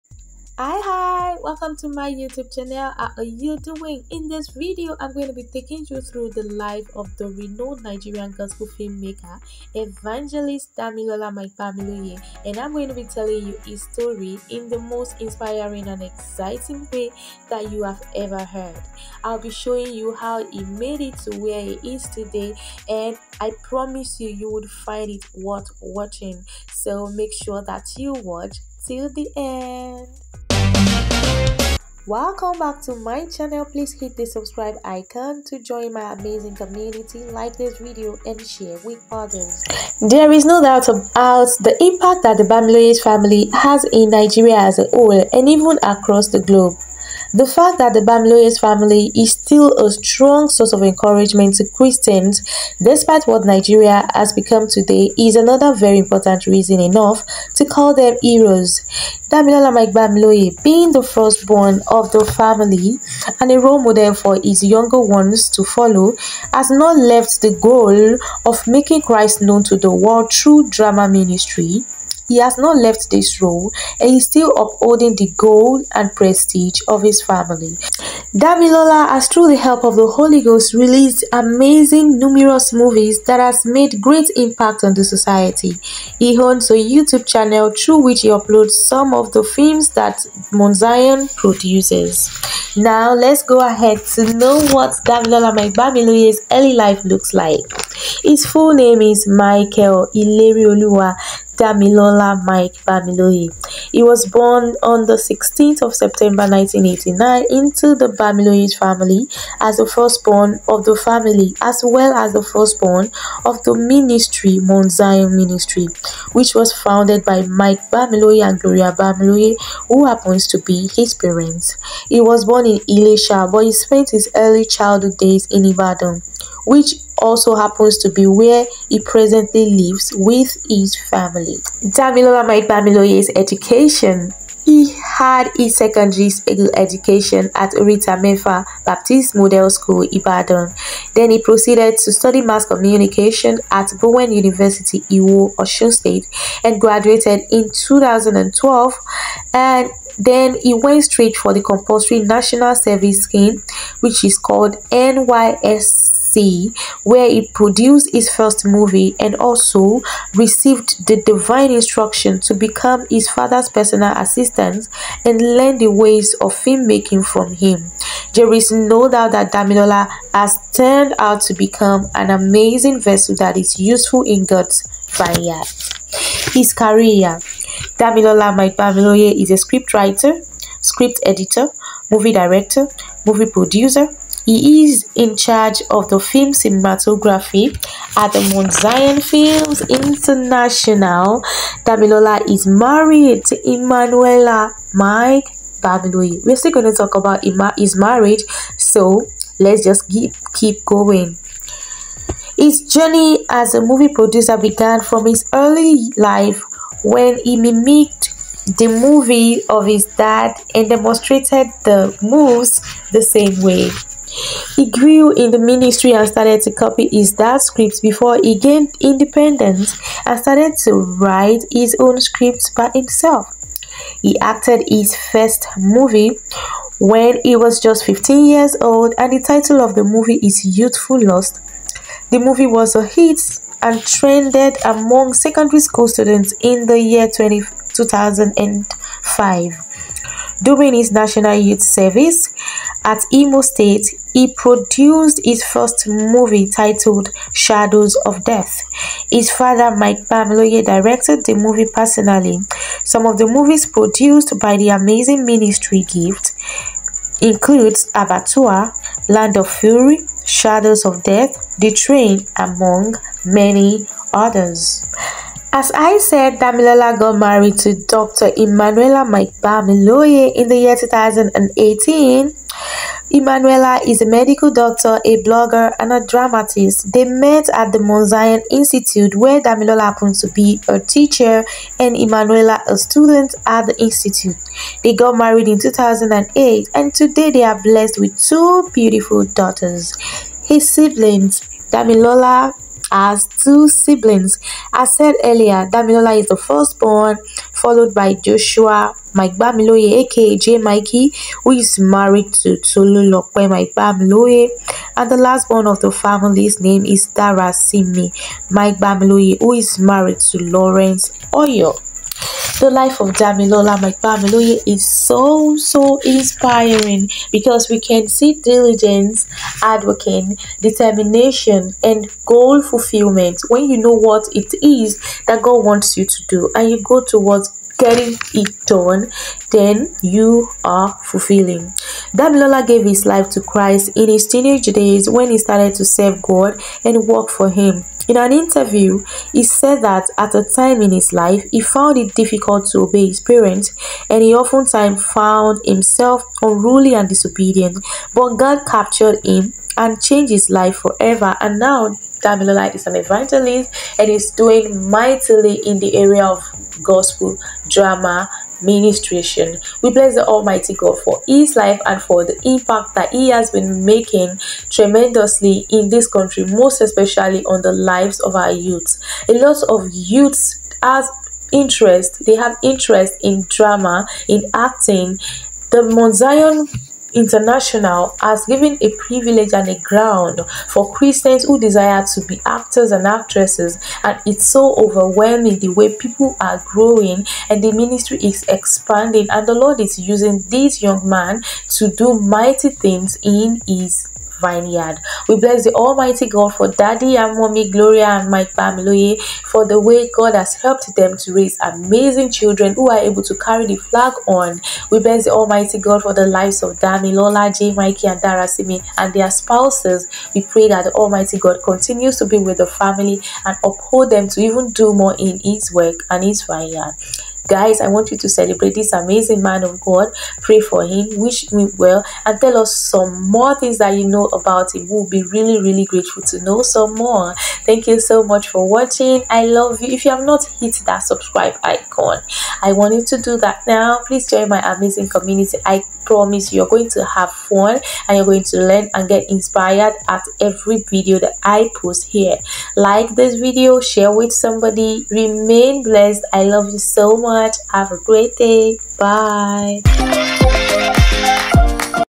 How Damilola Mike-Bamiloye Made It To The Top | A Story of Grace to Inspire You
In this video, I tell the story of Damilola Mike- Bamiloye  in the most amazing, inspiring and exciting way